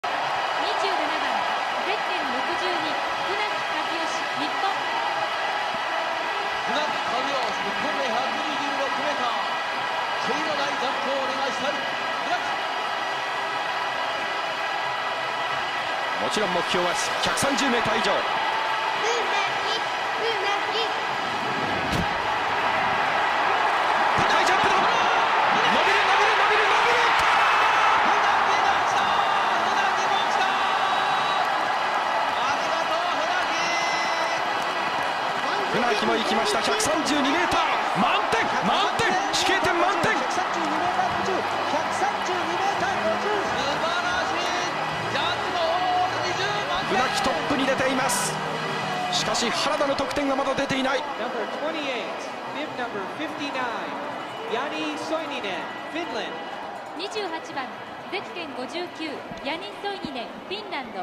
Funaki, Kazuyoshi - 132.5m - Nagano 1998 [16:9]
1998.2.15
Olympic Winter Games in Nagano
Ski-jumping K120 Individual
2nd round
Kazuyoshi Funaki - 132.5m - 2nd Round
16:9